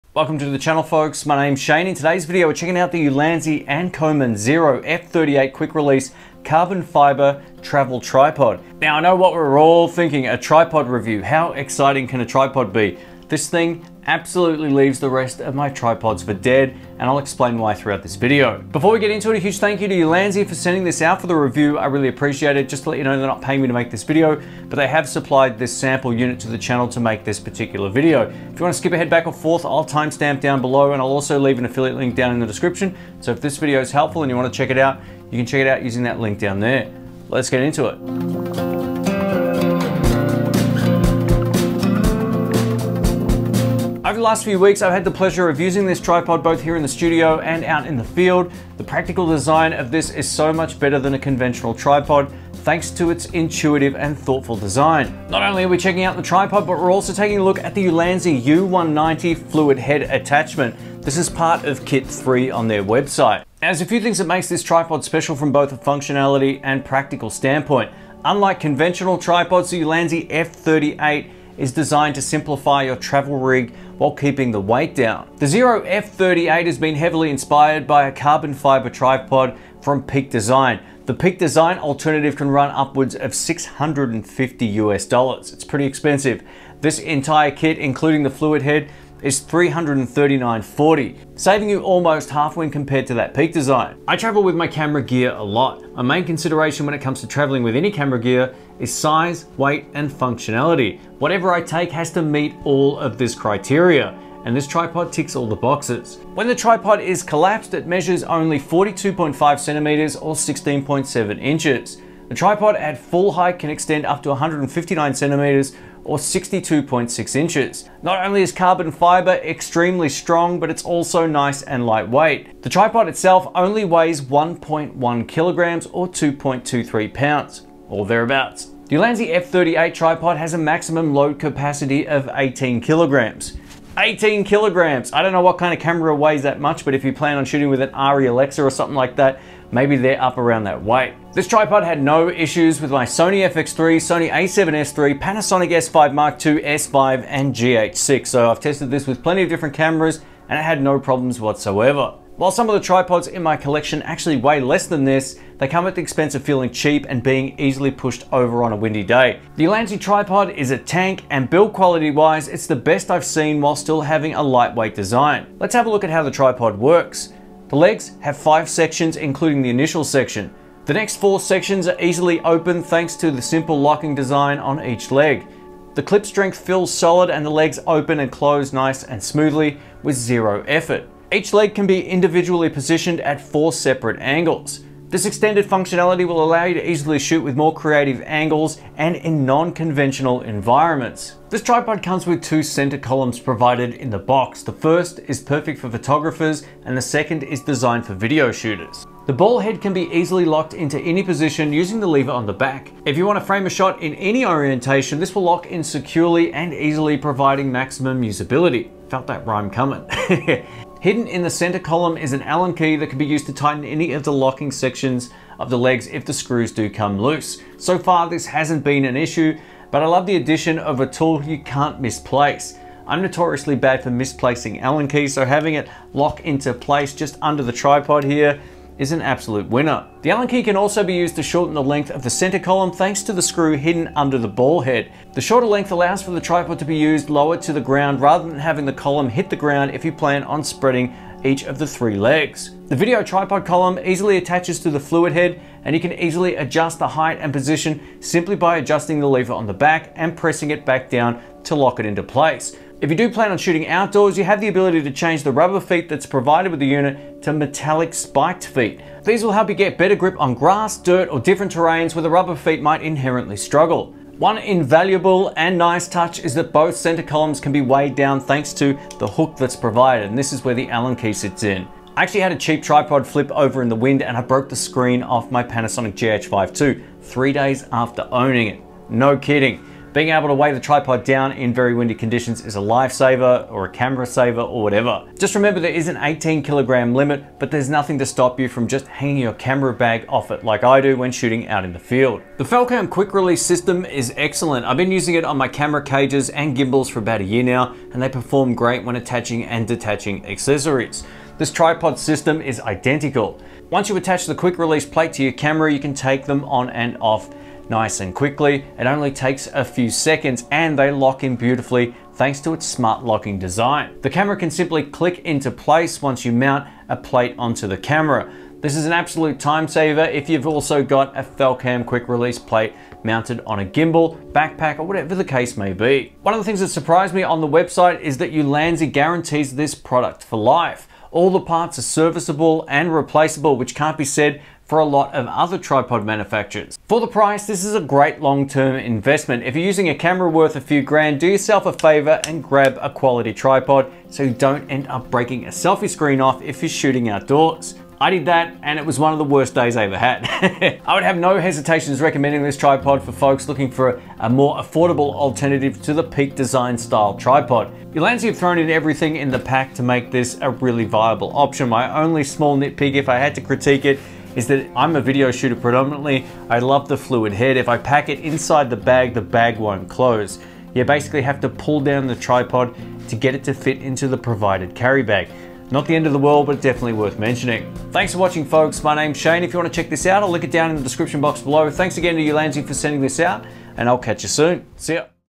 The Ultimate Compact & Lightweight Travel Tripod by Ulanzi Zero F38
🛒Amazon de
🛒Amazon Ca
🛒Amazon Uk
🛒Amazon AU

This is my full review of the Ulanzi & Coman Zero F38 Travel Tripod for Photography and Filmmaking. The Ulanzii F38 carbon fiber tripod is light, strong, and versatile for photographers and filmmakers.  This is the best travel tripod of 2023! Is the Ulanzi the best travel tripod ever? Possibly!? Let's get into the review.

Video Chapters:
00:00 - Ulanzi & Coman F38 Tripod Review
00:18 - Disclaimer (Product Provided)
00:43 - What we are checking out
01:03 - Ulanzi & Goman Tripod Overview
01:15 - Price
01:33 - Size, Weight, and Height
02:08 - Load Capacity
02:32 - Light & Strong
02:45 - Tripod Leg Exension
03:01 - Adjustable Leg Angles
03:30 - Included Tool (Hidden)
03:50 - Shortening the Center Column
04:12 - Rubber or Metal Spiked Feet Options
04:24 - Weighing Down Tripod (Max 18kgs)
05:36 - Pros & Cons Review

►Panasonic S5II
►Panasonic S Series Lenses
►Sony FX3
►Sony FX30
►Godox MoveLink II Wireless